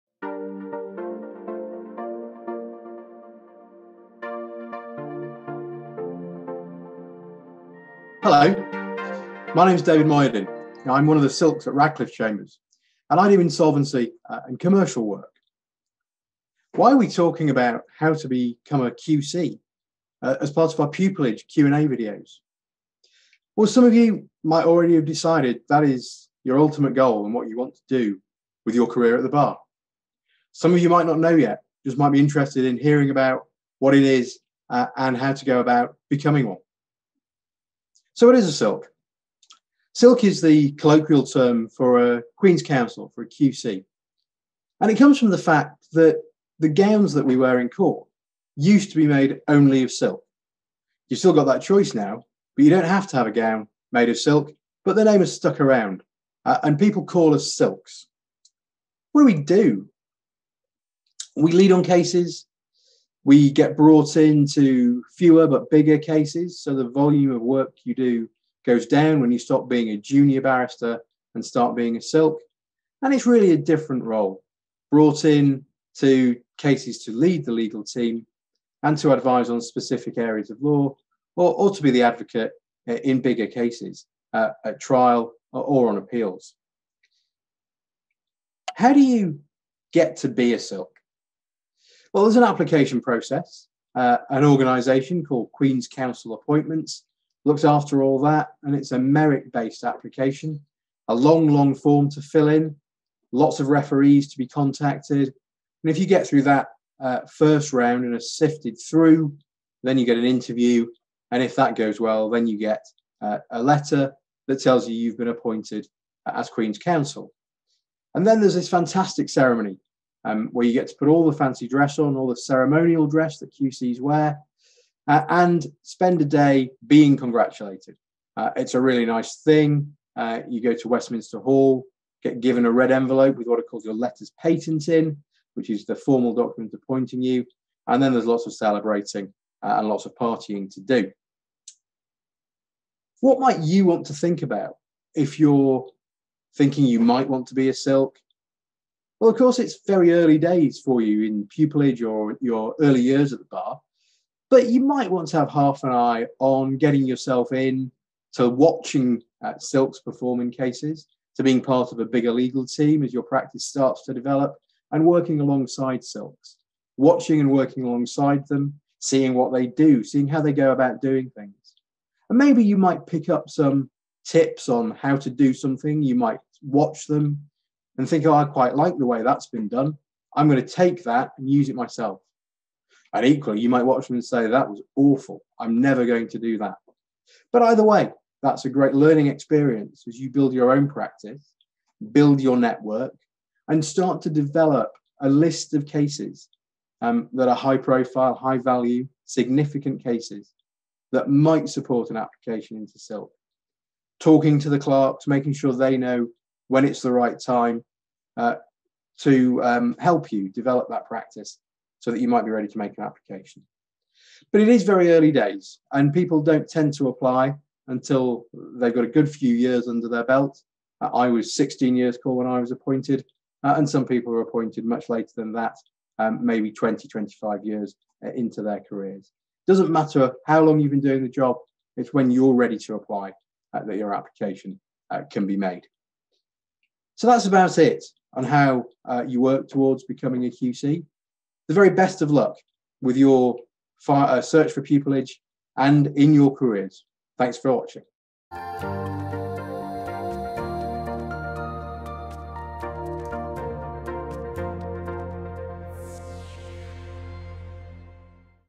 Pupillage Q&A: What should I be thinking about early in my career if I want to become a QC?
In this pupillage Q&A video David Mohyuddin QC explains what a QC is and how to become one.

David is a Queen's Counsel and mediator, practising in the fields of commercial and insolvency law.